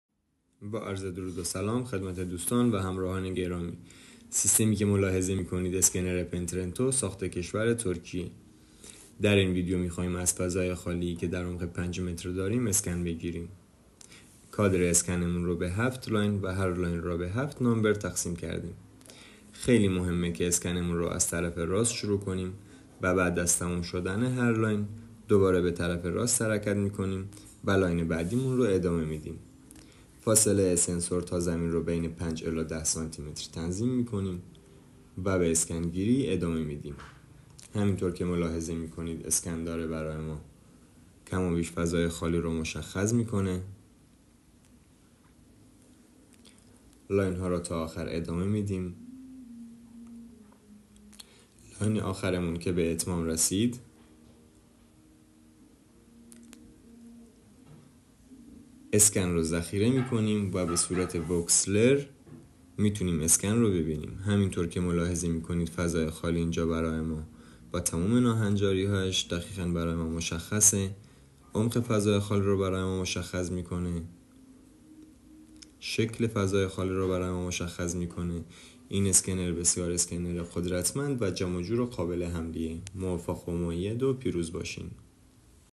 اسکنر سه بعدی Penetrator
مناسب فلزیابی و تونل یابی تا عمق ۱۸ متری